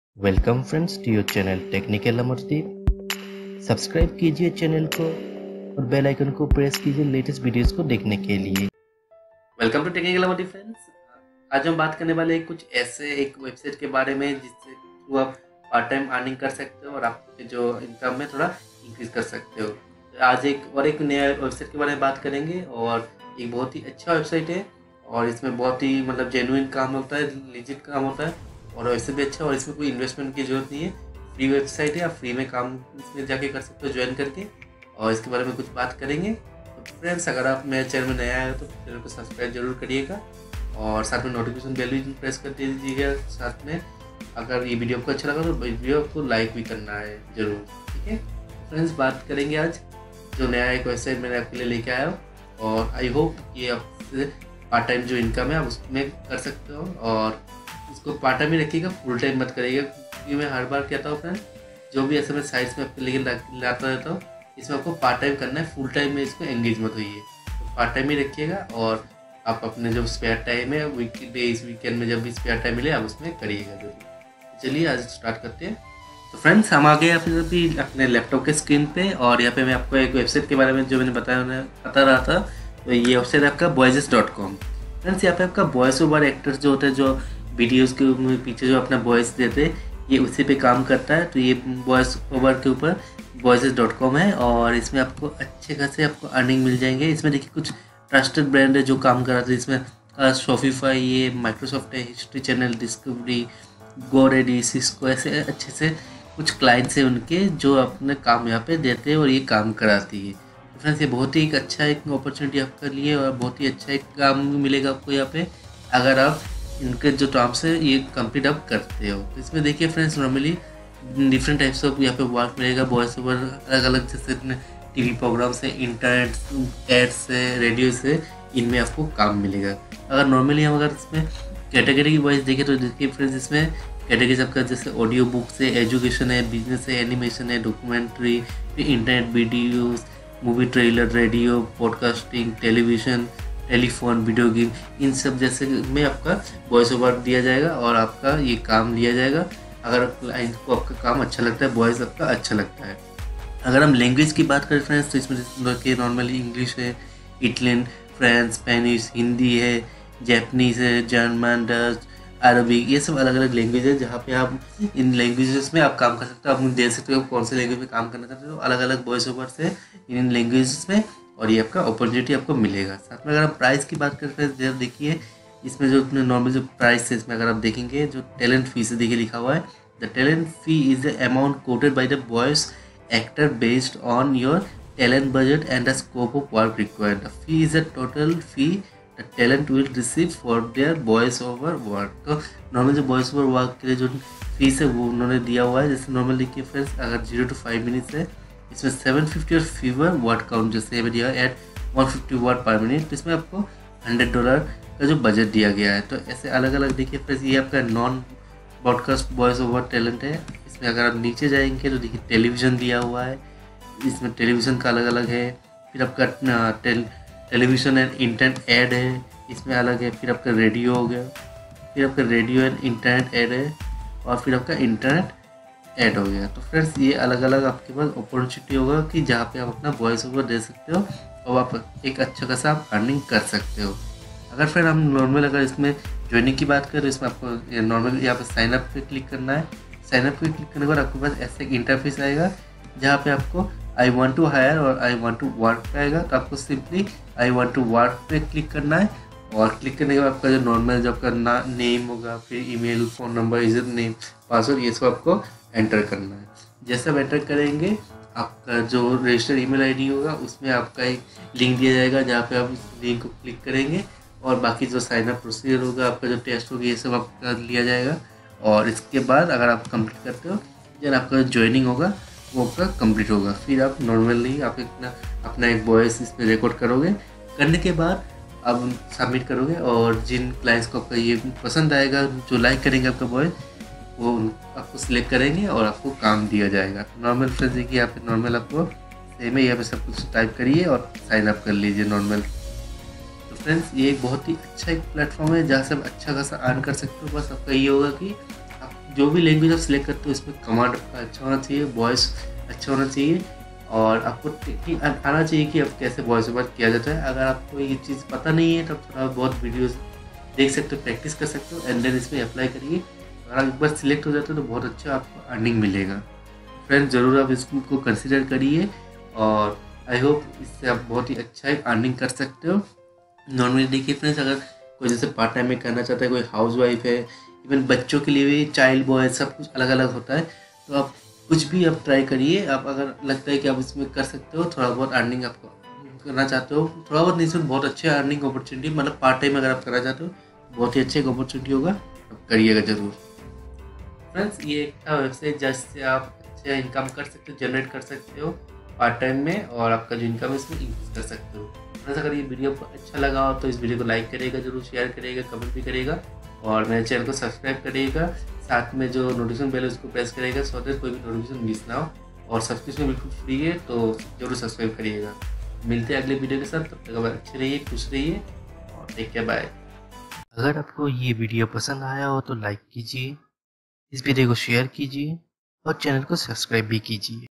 How to Earn Money Online Without Investment in Hindi
Hello Friends,
Welcome to your channel 'Technical Amardeep'

In this video, I have shared a source from where one can earn $100 by working part-time. So, watch the complete video to know the complete information.